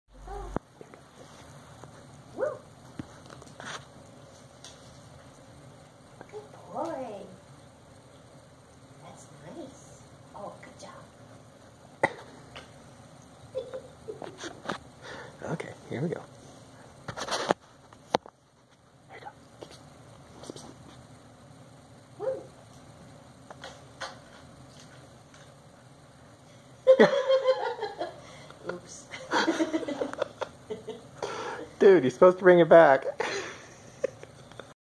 Stuart plays fetch, part 1.
But he distracts himself. Yes, he's a goof.